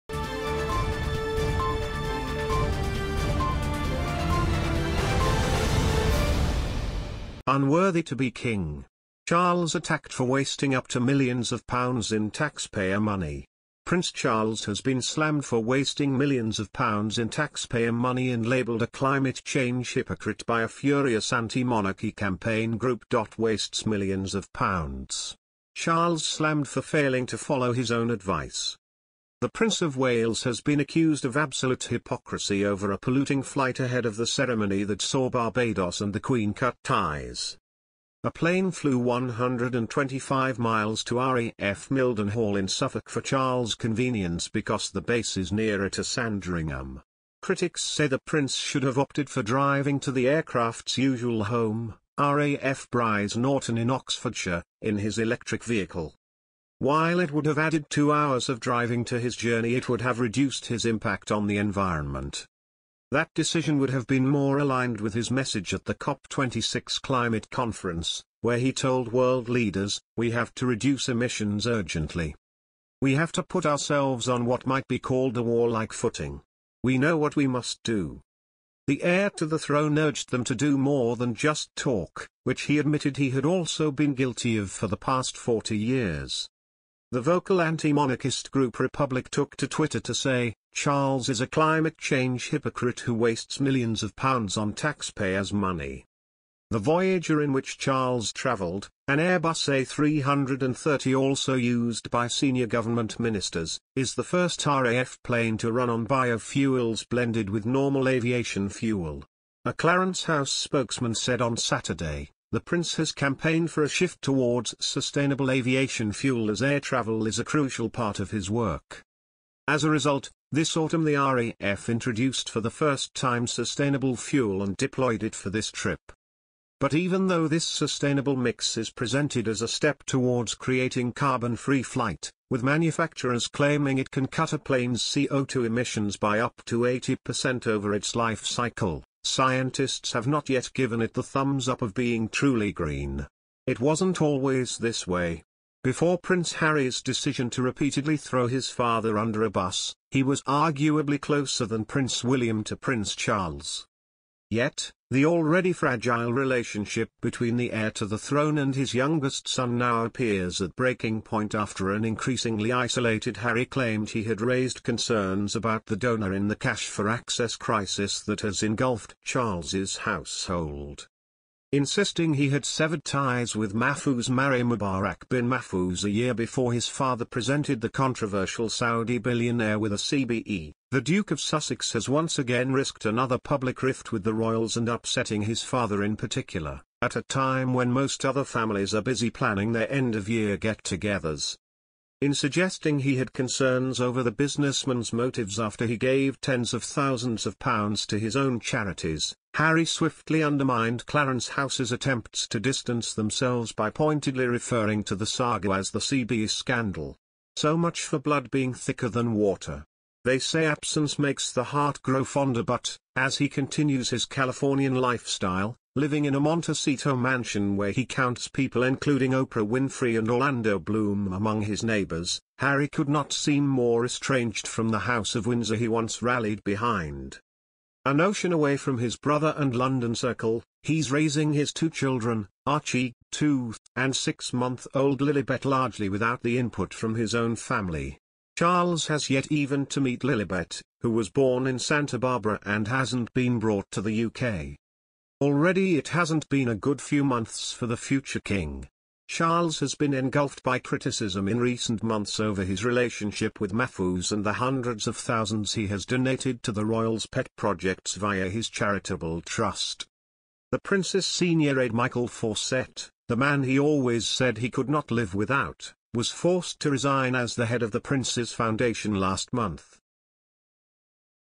UNWORTHY TO BE KING! Charles Attacked For WASTING Up To MILLIONS OF POUNDS In Taxpayer Money
PRINCE Charles has been slammed for "wasting millions of pounds in taxpayer money" and labelled a "climate change HYPOCRITE" by a furious anti-monarchy campaign group.'Wastes millions of pounds!' Charles slammed for failing to follow his OWN advice
The Prince of Wales has been accused of "absolute hypocrisy" over a polluting flight ahead of the ceremony that saw Barbados and the Queen cut ties.

A plane flew 125 miles to RAF Mildenhall in Suffolk for Charles' convenience because the base is nearer to Sandringham. Critics say the prince should have opted for driving to the aircraft’s usual home, RAF Brize Norton in Oxfordshire, in his electric vehicle.